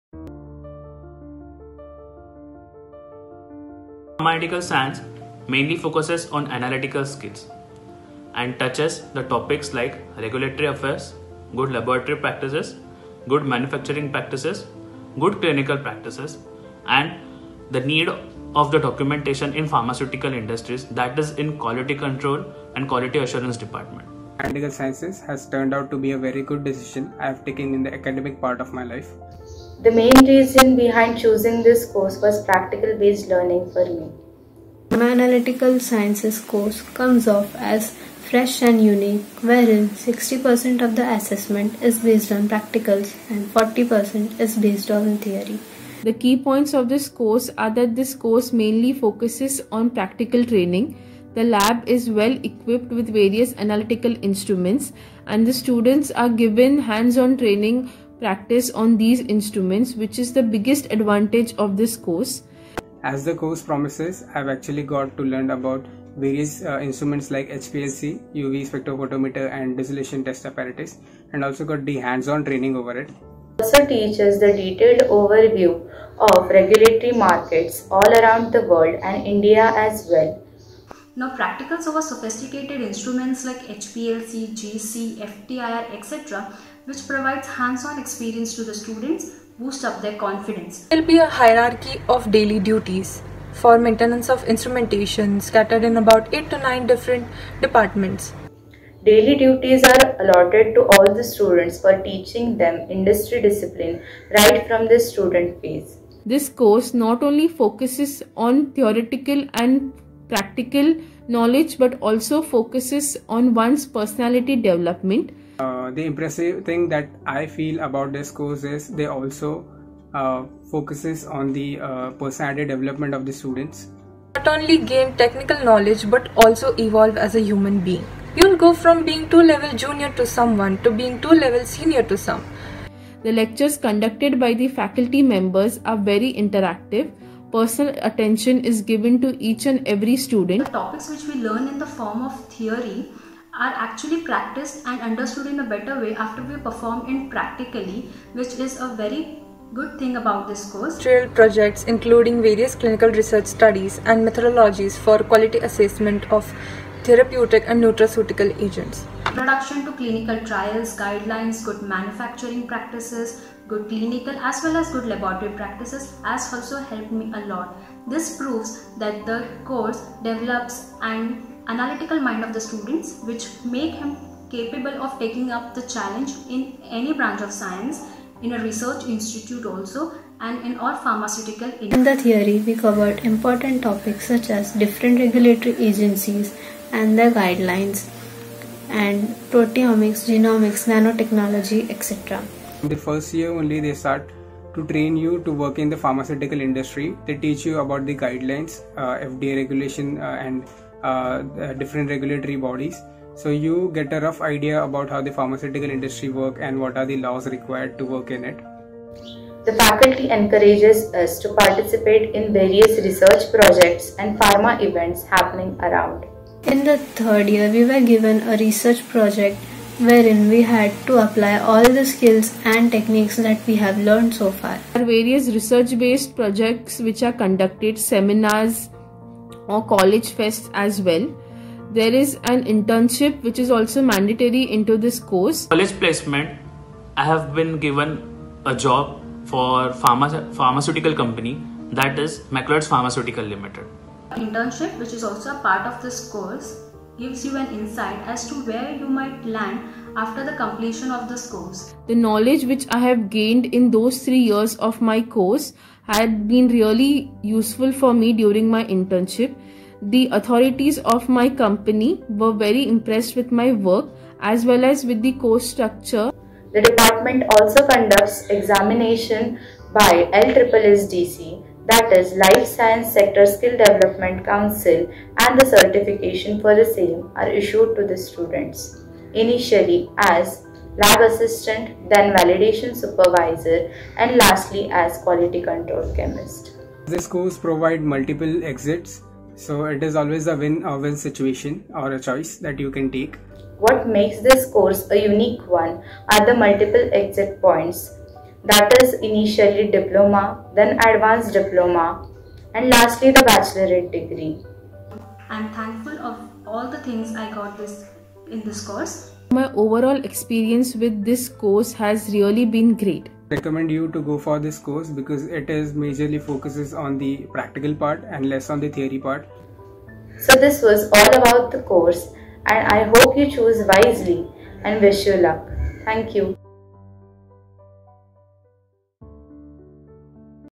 ALUMNI VIEWS on BVoc PAS 2020
The video provides the responses of former students who have completed the course in BVoc - Pharma Analytical Sciences. The video is a record of student testimonials of the course and will serve as a good guidance for aspiring students.